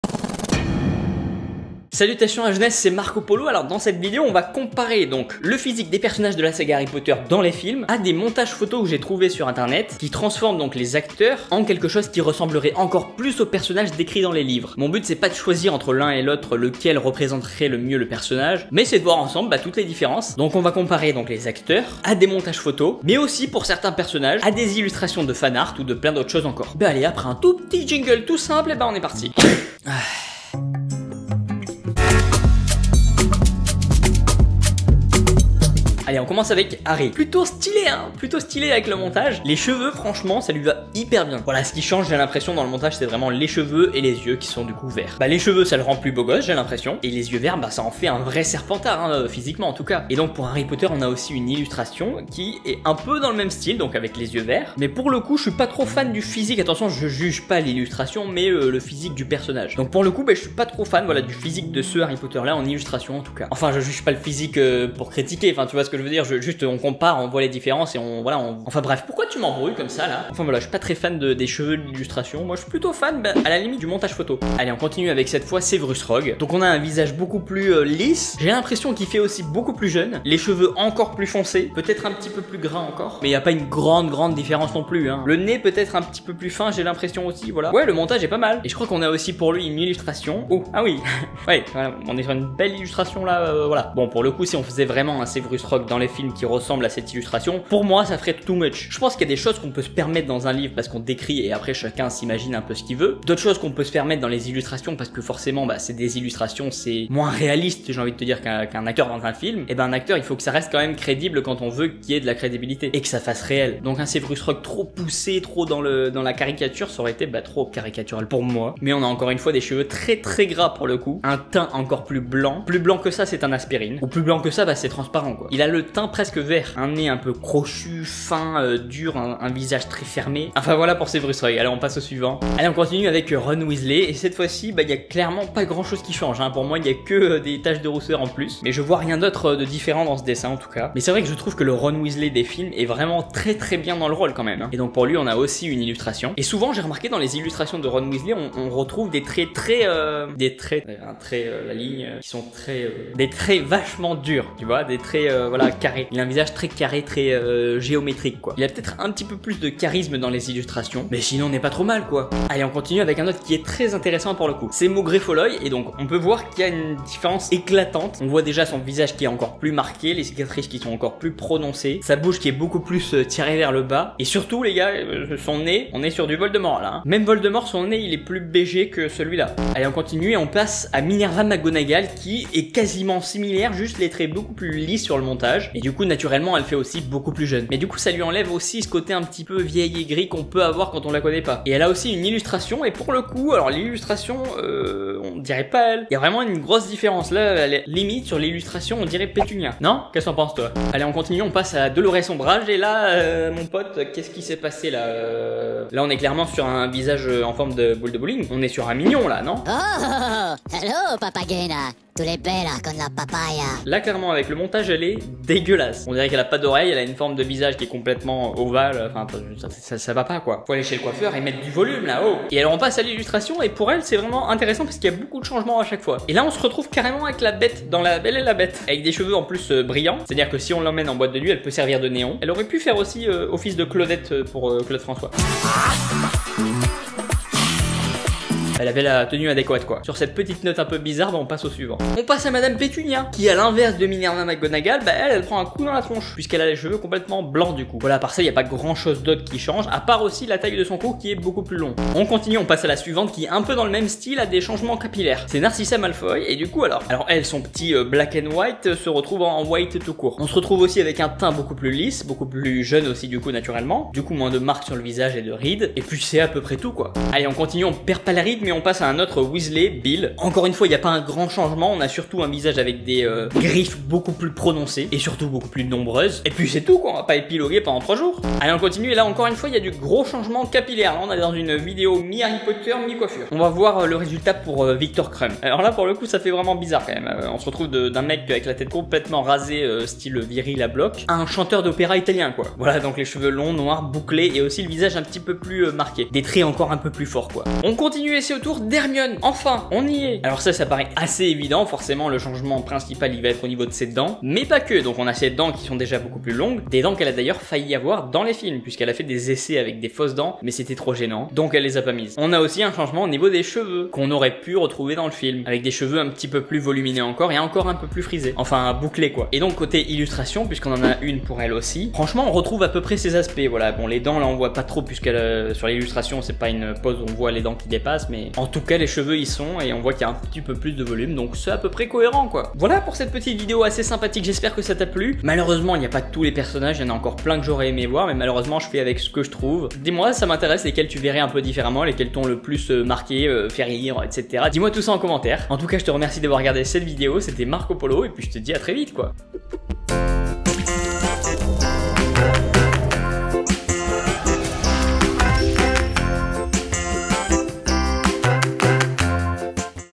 HARRY POTTER : PERSONNAGES LIVRE VS FILM
Salutations la jeunesse, c’est Marco Polo, j’ai découvert sur internet, des montages photos des personnages Harry Potter des films modifiés pour qu’ils ressemblent à ceux décrits dans les livres. J’ai trouvé ça hyper original et je suis sûr que ça vous plaira ! Donc ensemble, découvrons les personnages de la saga Harry Potter des films vs les personnages des livres Harry Potter !

Bon visionnage !
Marco Polo